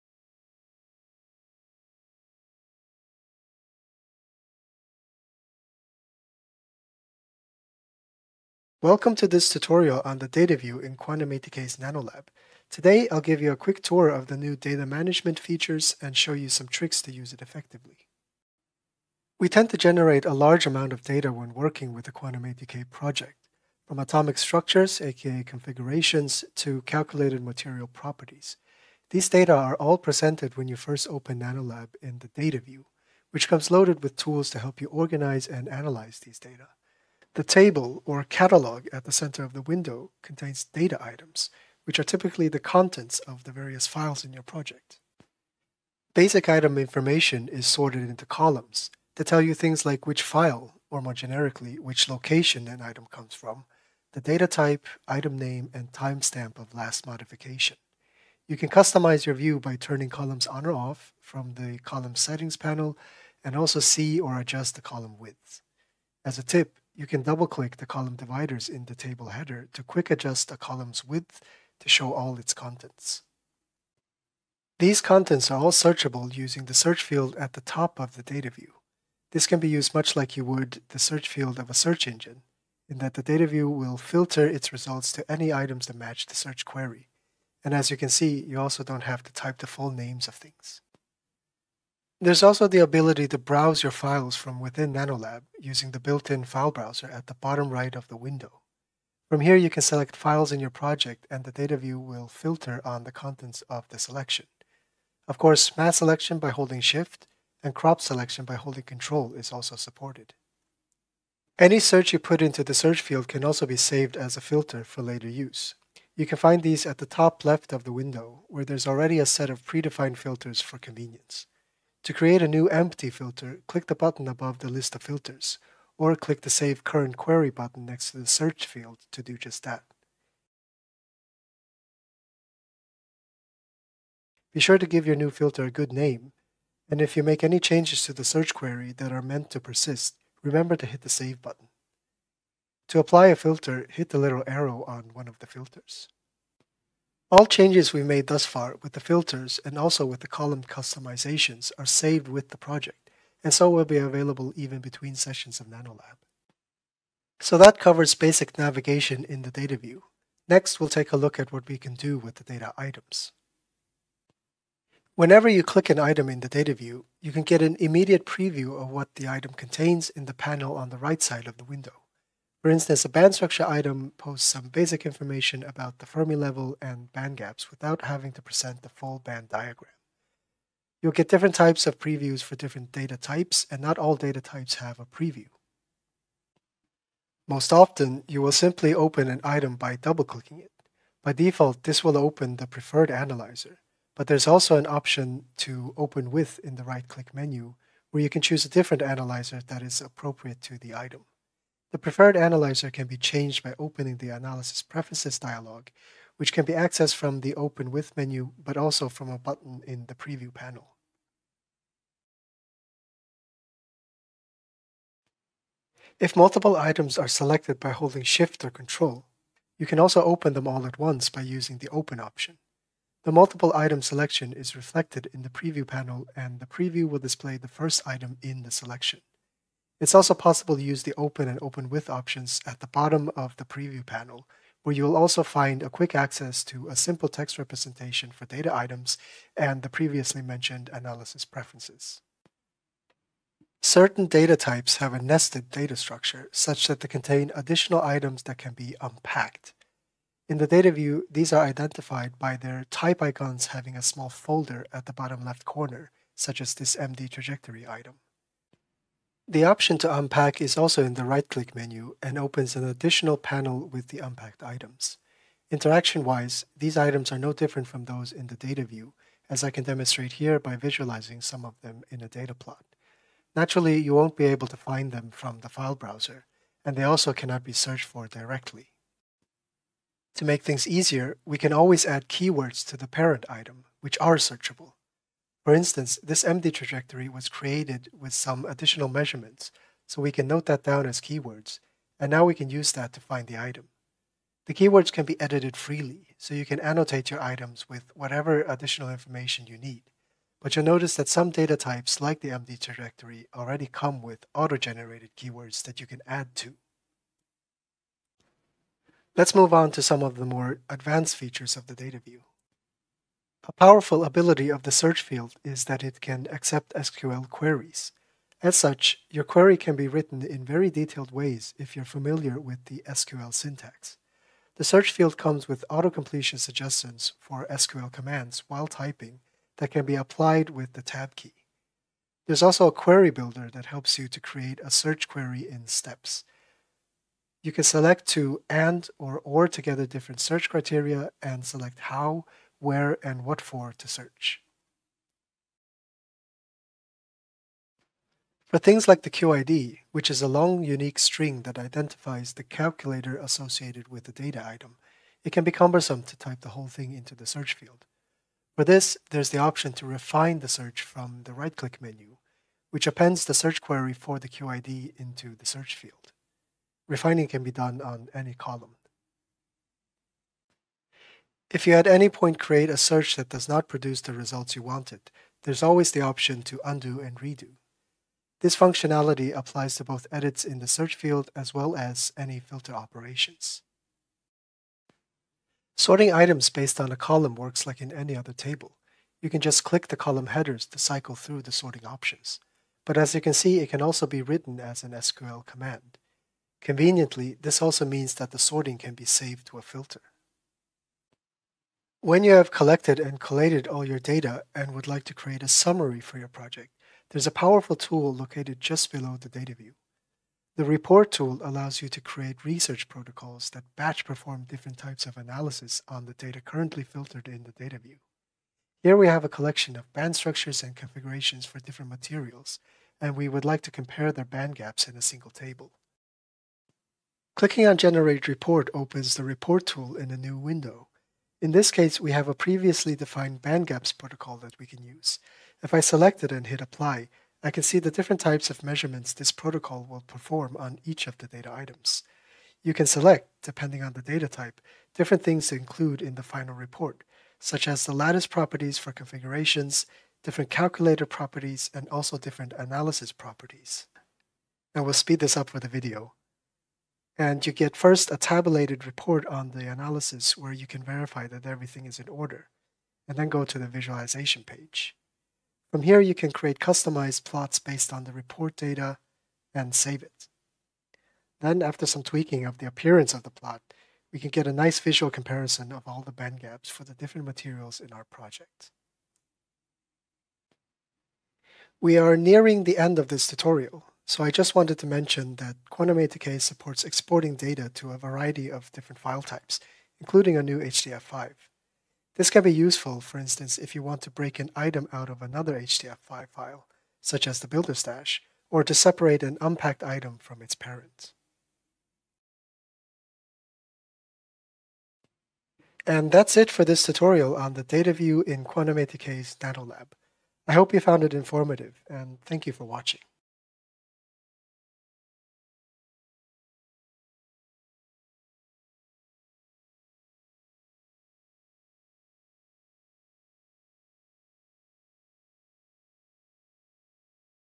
QuantumATK NanoLab GUI: Data-Intensive Atomistic-Scale Modeling
Benefit from the QuantumATK NanoLab GUI layout, enabling to work efficiently with data intensive atomic-scale modeling projects based on multiple simulations.